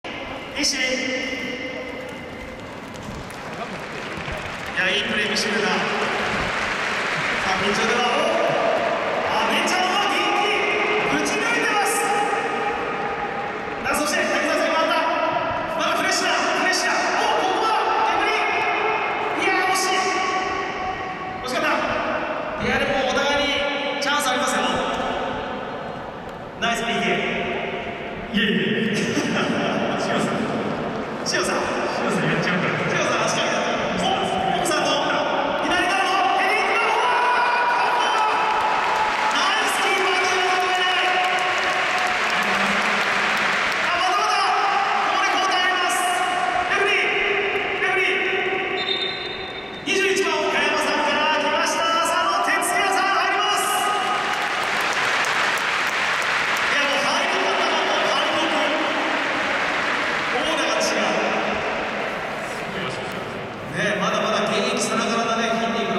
小倉のクロス→玉田ヘディングシュートは･･･！ グランパス30周年レジェンドマッチvs愛知県中学校フットボールアカデミー 2022/5/14
前半12分、阿部翔平（6番）のサイドチェンジを受け、小倉隆史（19番）が右サイドからクロスボールを上げる。そこに走り込んだ玉田圭司は見事頭に合わせるも、そのボールはポストに弾かれる。